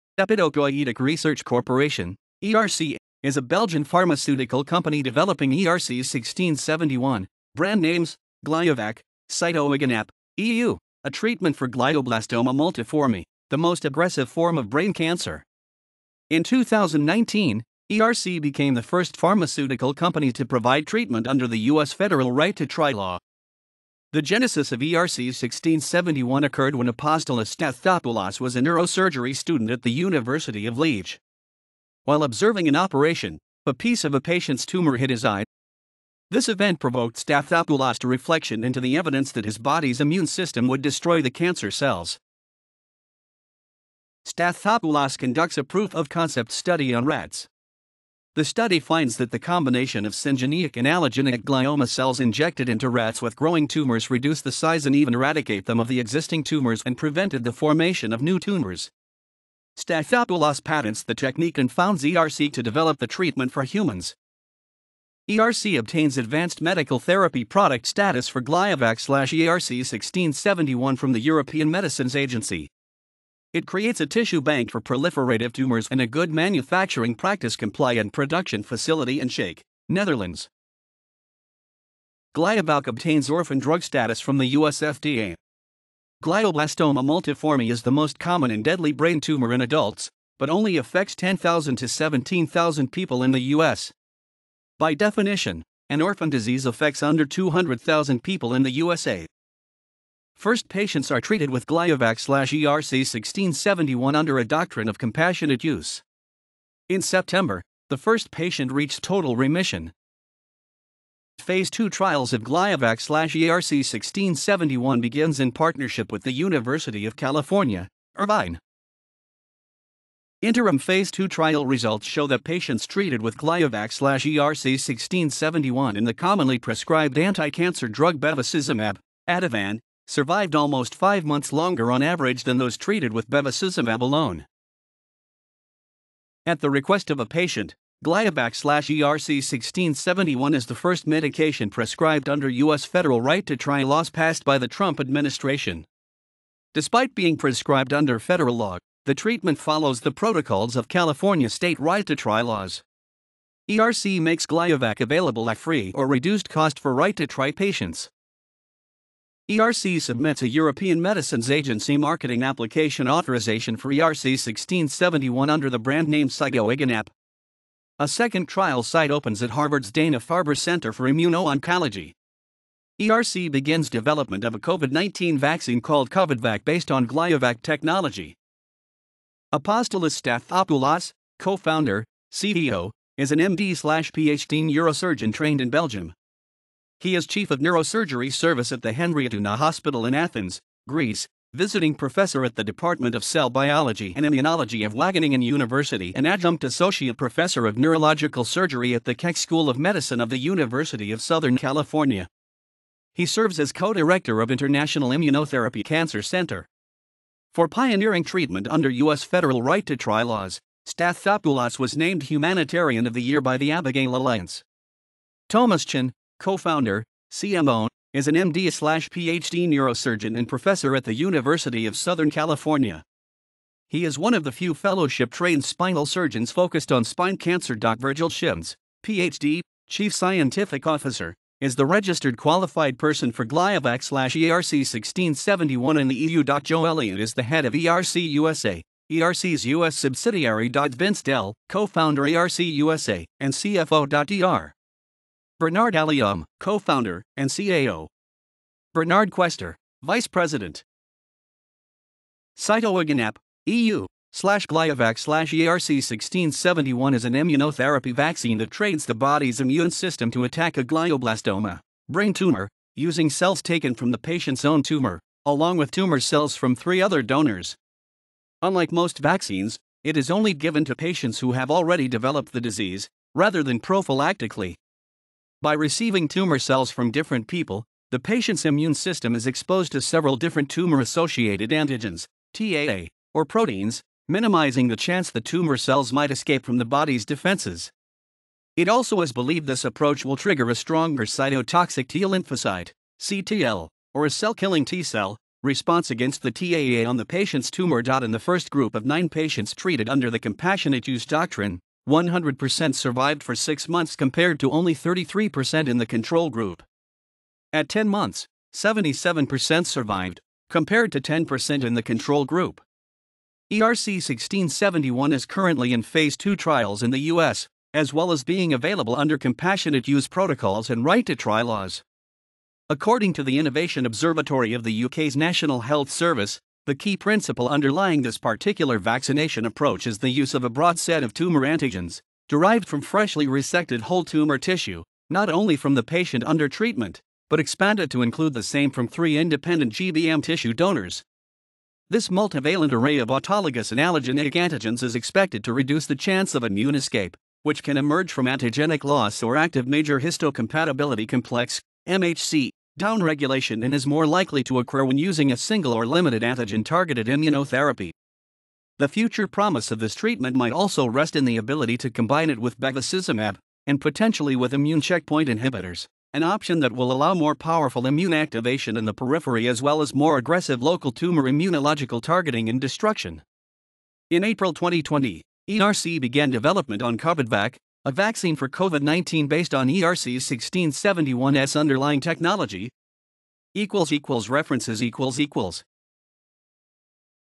Epitopoietic Research Corporation
Epitopoietic Research Corporation (ERC) is a Belgian Pharmaceutical company developing ERC1671 (brandnames: Gliovac, Sitoiganap (EU)), a treatment for Glioblastoma multiforme, the most aggressive form of brain cancer. In 2019, ERC became the first pharmaceutical company to provide treatment under the US Federal Right-to-try law.